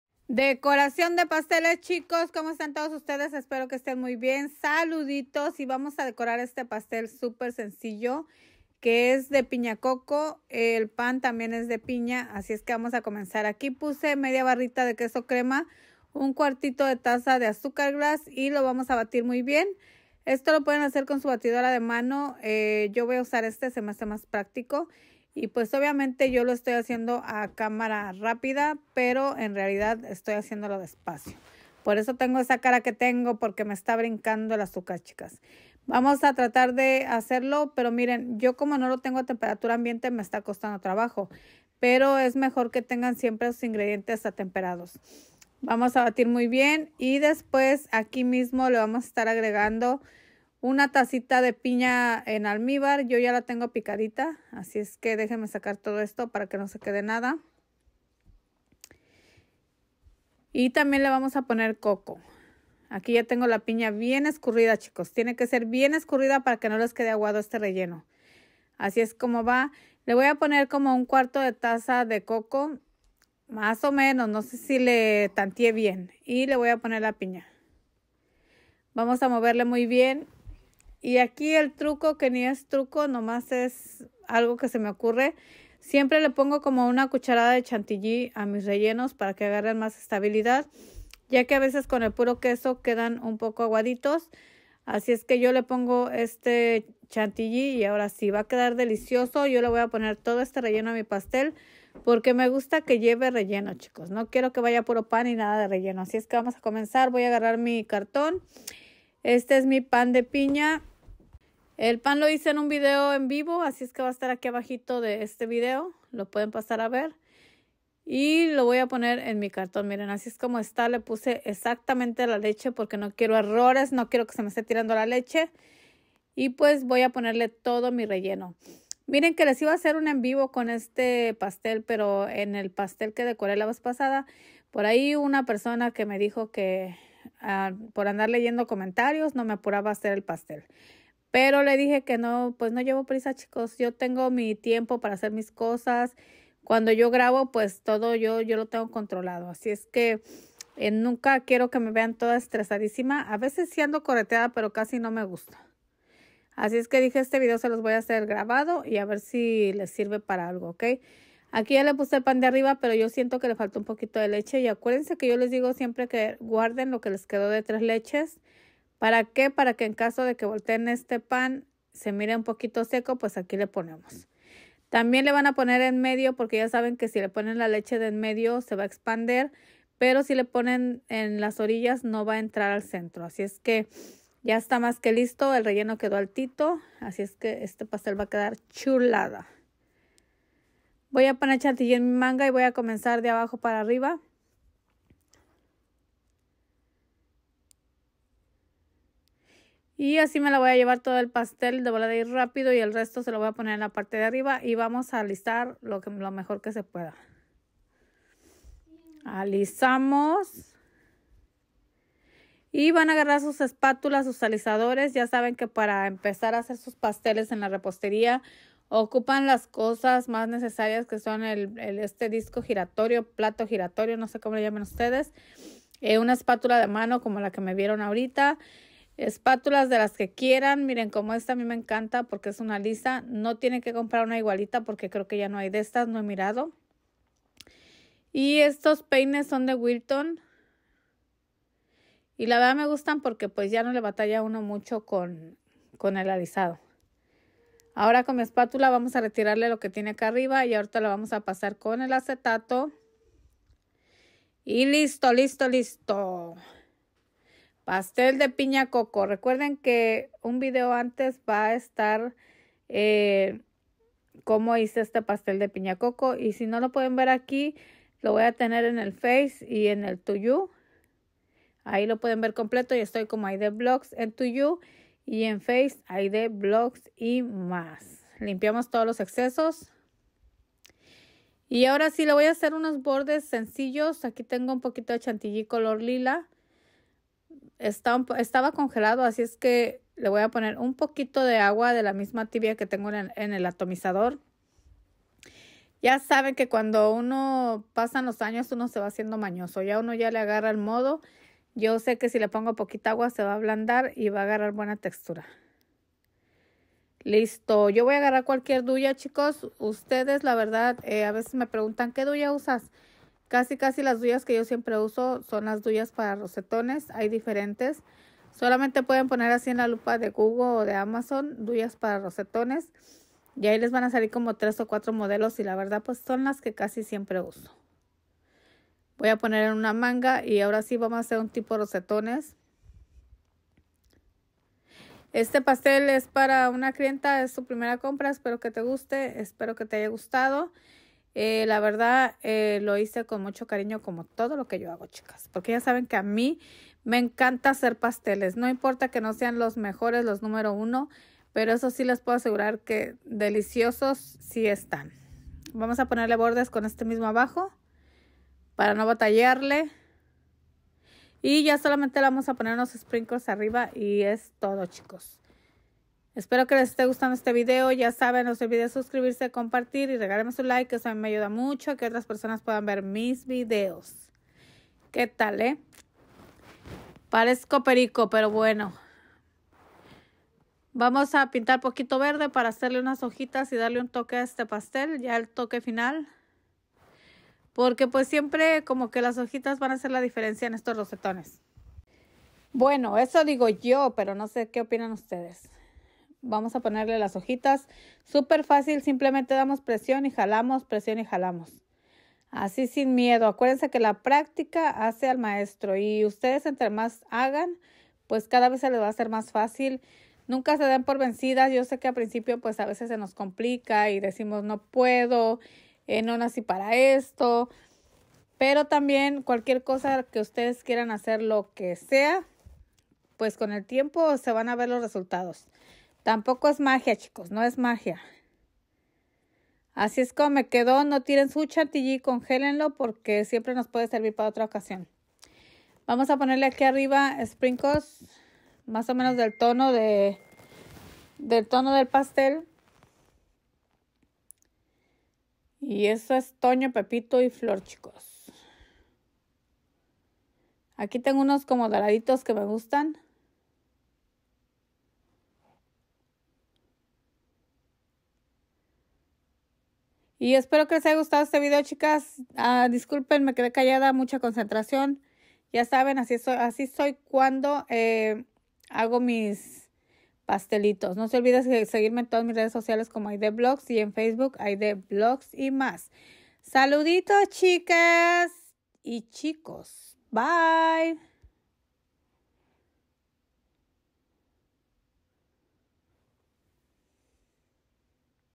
Decorando un pastel 🍰 de piña coco 🍍🥥
Aquí les dejo una decoración súper sencilla pero la verdad es que pastel me encantó porque es uno de mis favoritos se los recomiendo que lo hagan ya que queda delicioso aquí otro video abajo de este tengo la preparación de cómo hacer el pan para este pastel ￼